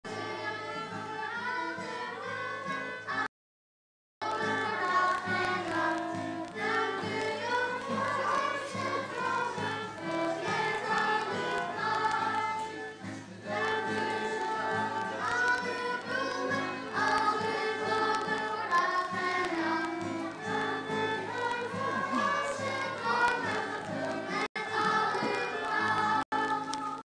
A play of 2Y
Childrens' act of St Francis of Assissi

Second of my raw camera films.